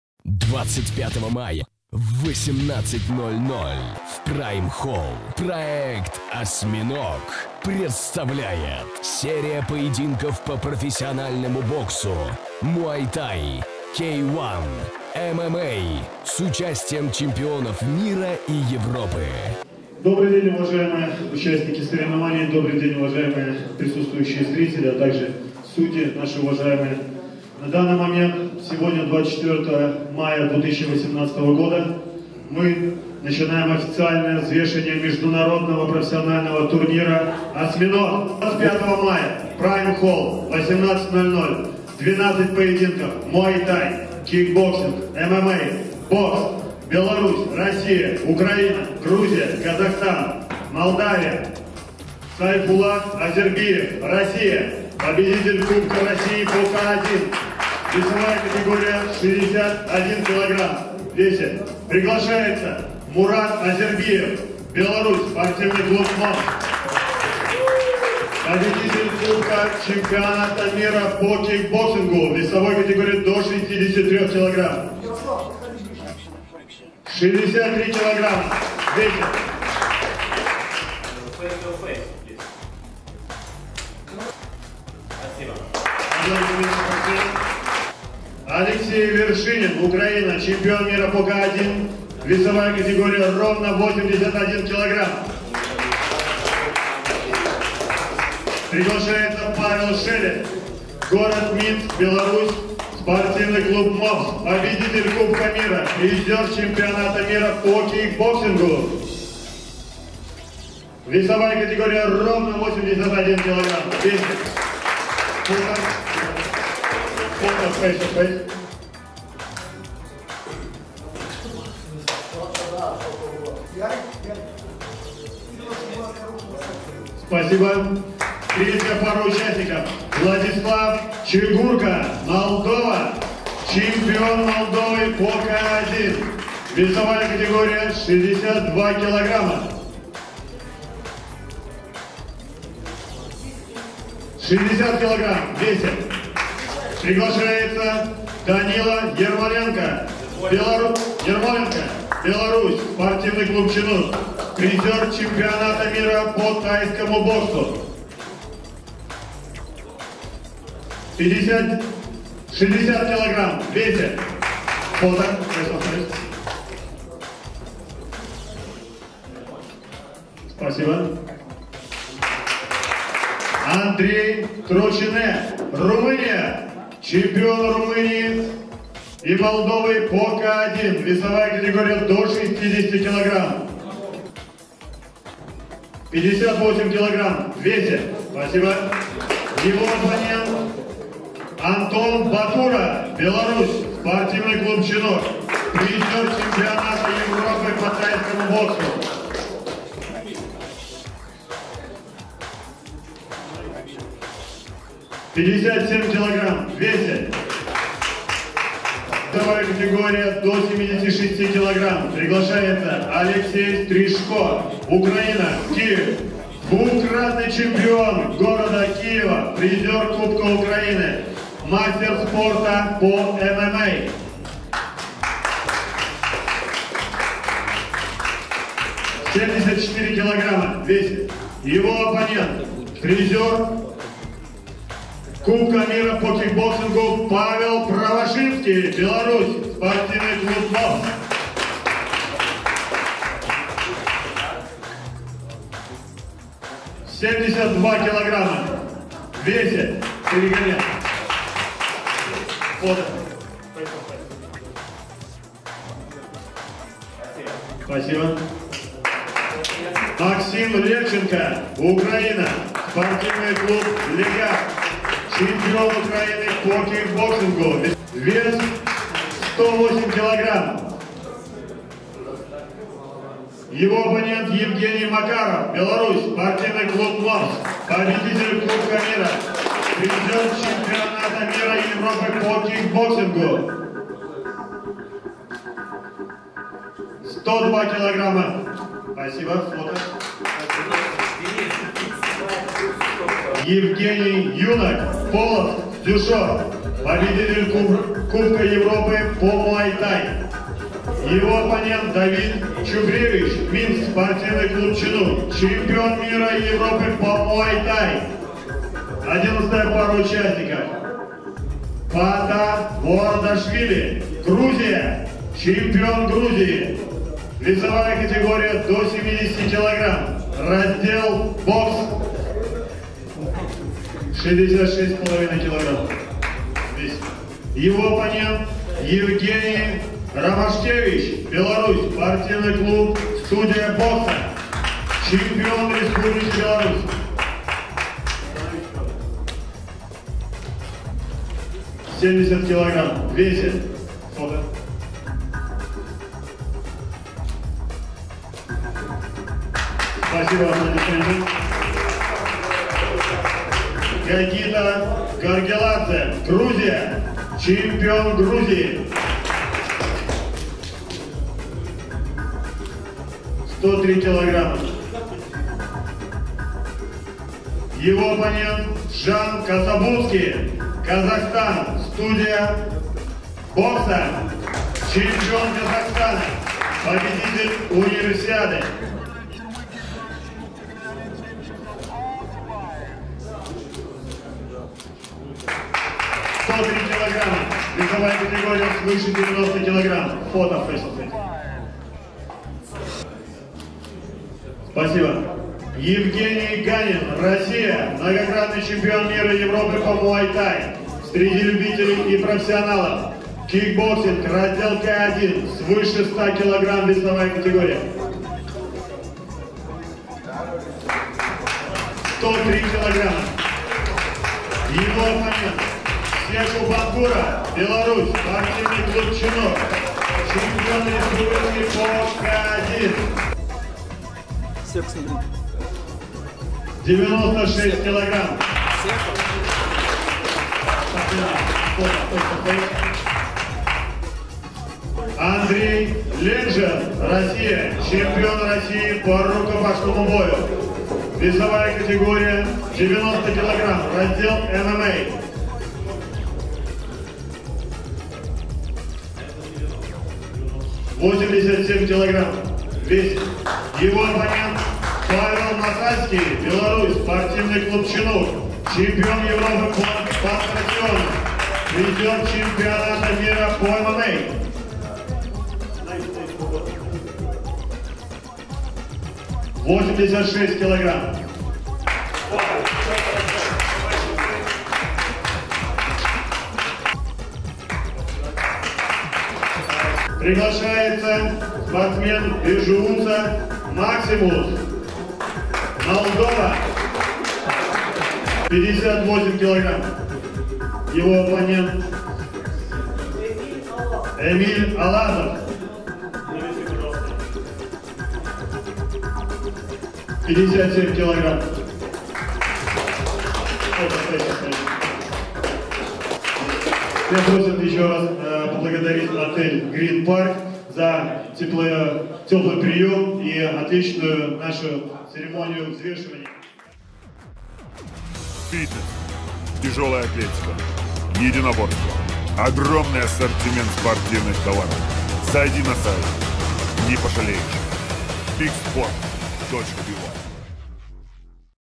Взвешивание перед бойцовским турниром "ОСЬМИНОГ" СПОРТ РБ
Взвешивание спортсменов перед Международным турниром «Осьминог»
Рекомендуем СПОРТИВНЫЕ ТОВАРЫ на OZON
       ПОХУДЕТЬ быстро благодаря этому термо костюм сауна

     ФАЙТБОЛ. Супер тренажер

Инста youtube_bigsport
Тик Ток youtube_bigsport